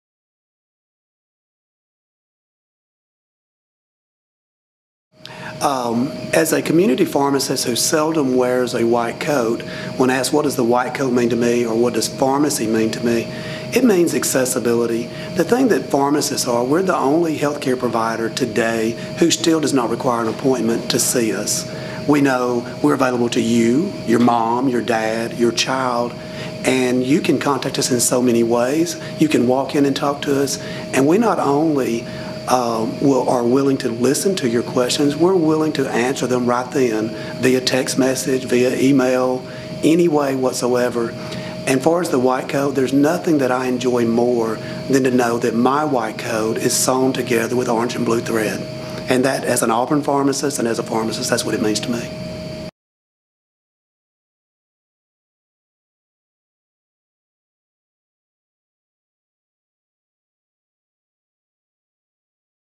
What the White Coat Means to Me: Bobby Giles
Throughout the month of October, Harrison School of Pharmacy students, faculty and alumni will recognize American Pharmacists Month by sharing what the white coat means to them and what it means to be a pharmacist.